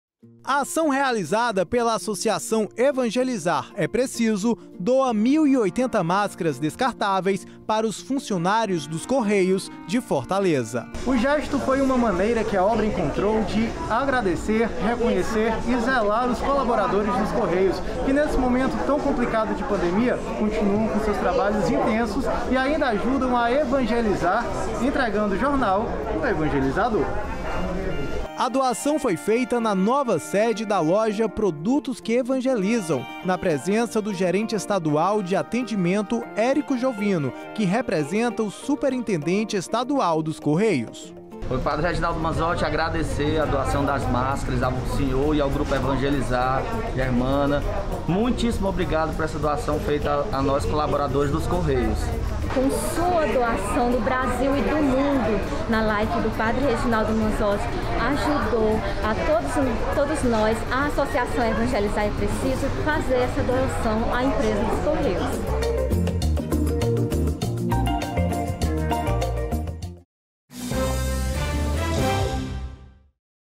Associação Evangelizar É Preciso doa máscaras aos funcionários dos Correios de Fortaleza
Vamos juntos?

JOR21VT347ENTREGAMASCARASCORREIOSFORTALEZA